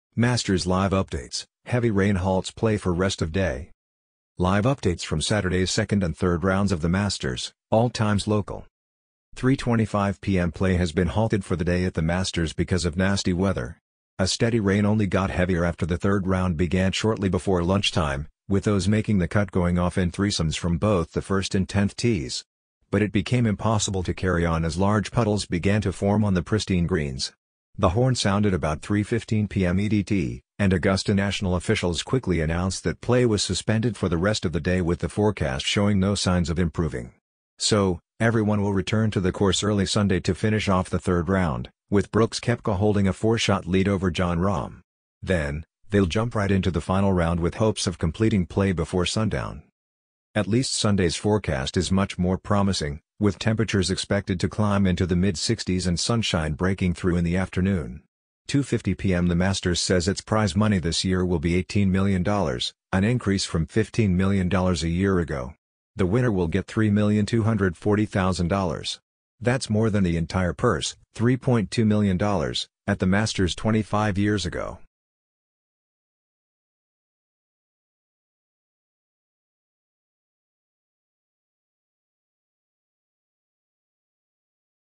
Masters Live Updates | Heavy rain halts play for rest of day
Live updates from Saturday’s second and third rounds of the Masters (all times local):
3:25 p.m.
Play has been halted for the day at the Masters because of nasty weather.
A steady rain only got heavier after the third round began shortly before lunchtime, with those making the cut going off in threesomes from both the first and 10th tees.
But it became impossible to carry on as large puddles began to form on the pristine greens.
The horn sounded about 3:15 p.m. EDT, and Augusta National officials quickly announced that play was suspended for the rest of the day with the forecast showing no signs of improving.
So, everyone will return to the course early Sunday to finish off the third round, with Brooks Koepka holding a four-shot lead over Jon Rahm. Then, they’ll jump right into the final round with hopes of completing play before sundown,
At least Sunday’s forecast is much more promising, with temperatures expected to climb into the mid-60s and sunshine breaking through in the afternoon.
2:50 p.m.
The Masters says its prize money this year will be $18 million, an increase from $15 million a year ago. The winner will get $3,240,000. That’s more than the entire purse ($3.2 million) at the Masters 25 years ago.